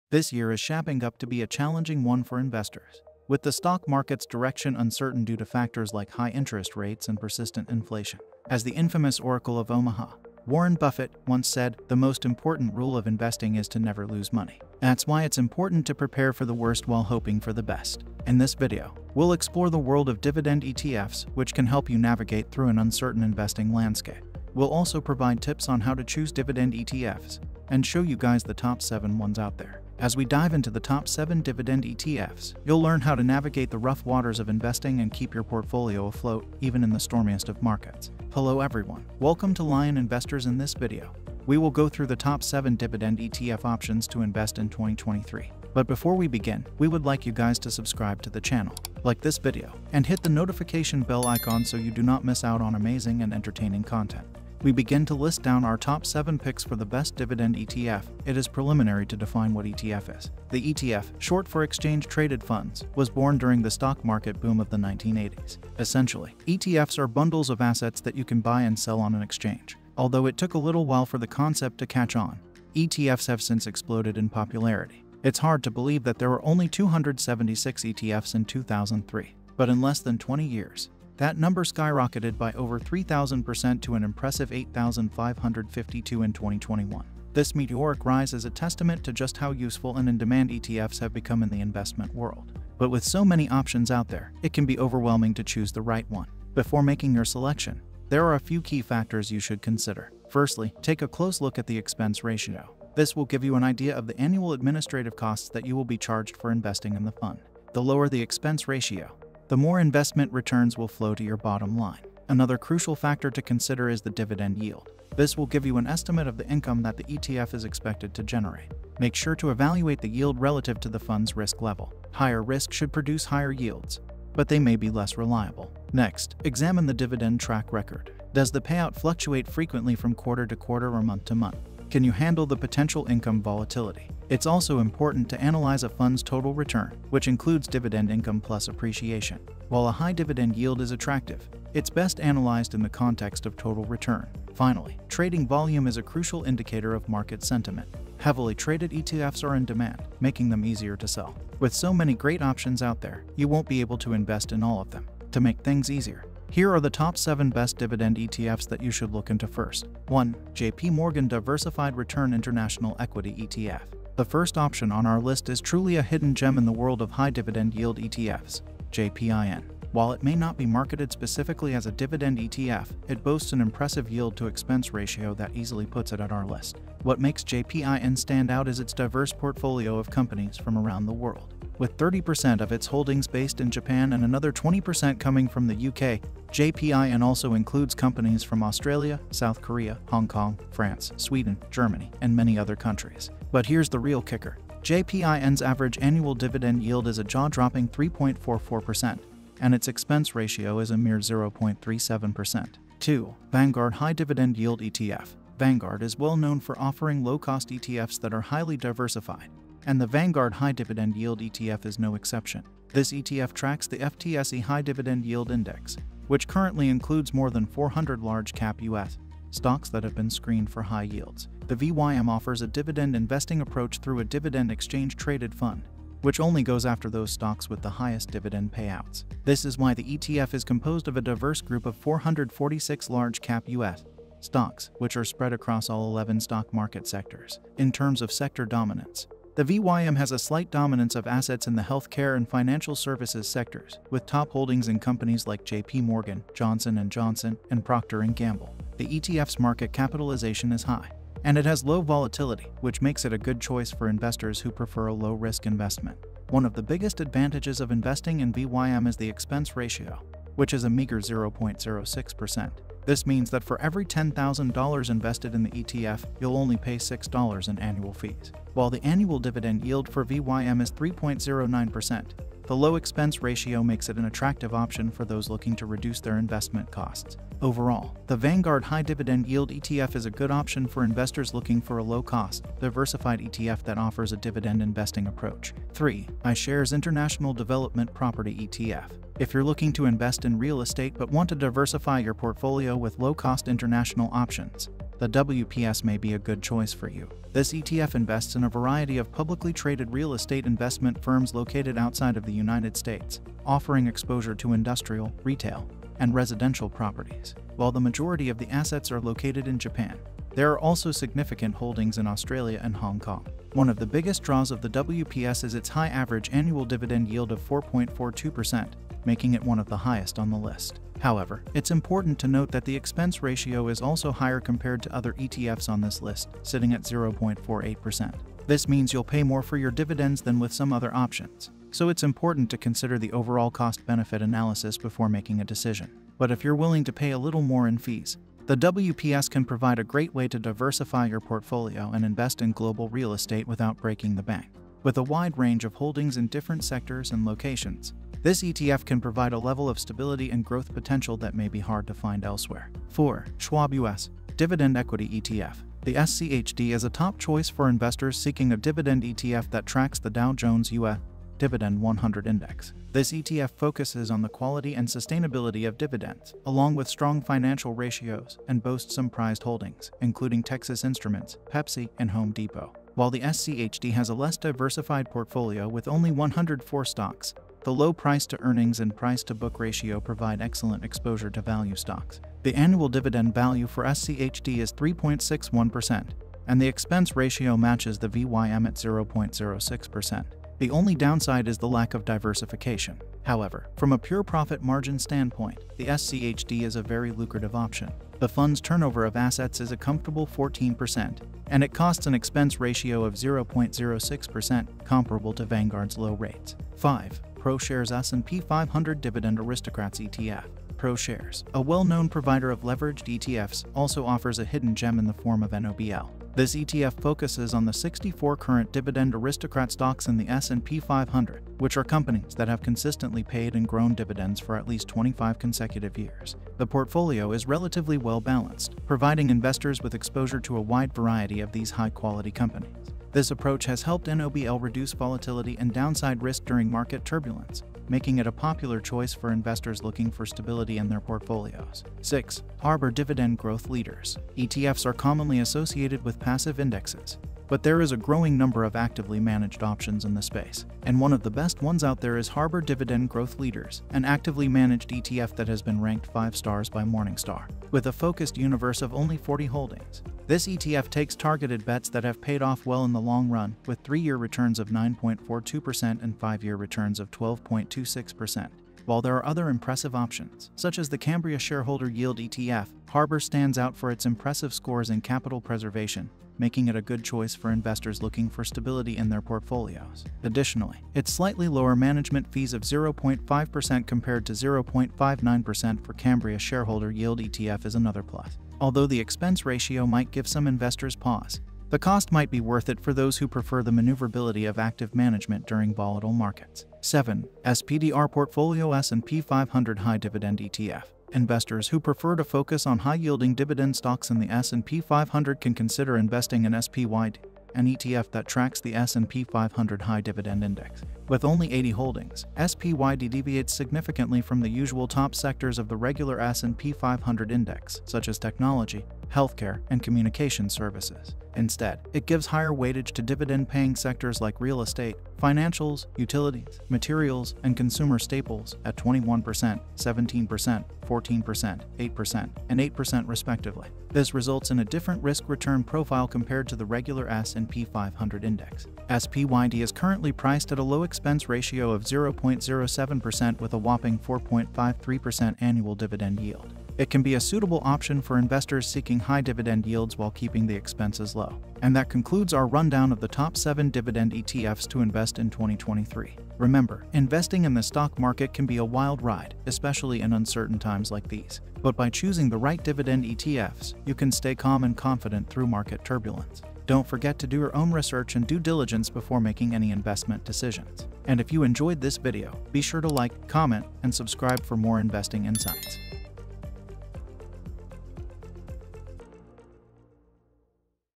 Top 7 Dividend ETF to invest in 2023
Lion Investors is the Channel for Stock Market, Real Estate and Financial investment for passive income

In this video, we'll discuss the top 7 Dividend
ETFs to invest in 2023, including Vanguard
Dividend Appreciation ETF, iShares Select
Dividend ETF, Schwab U.S. Dividend Equity ETF,
SPDR S&P Dividend ETF, iShares Core Dividend
Growth ETF, WisdomTree U.S. Quality Dividend
Growth Fund, and Invesco Dividend Achievers
ETF. These ETFs provide a stable source of
income for your portfolio, and we'll highlight
their performance, expense ratios, and dividend
yields. Don't miss out on this opportunity to
invest in these top-performing Dividend ETFs!